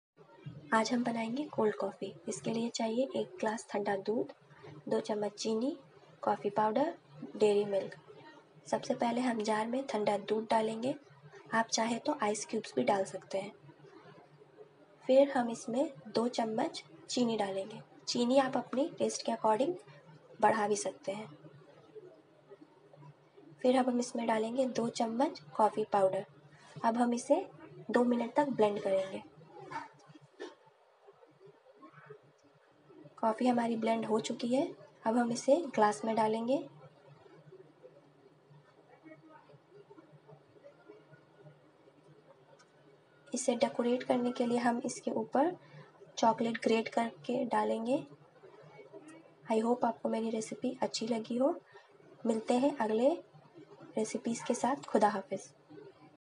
Instent cold coffee
1 small glass of chilled milk
2 tsp sugar
2 tsp coffee powder
1 diary milk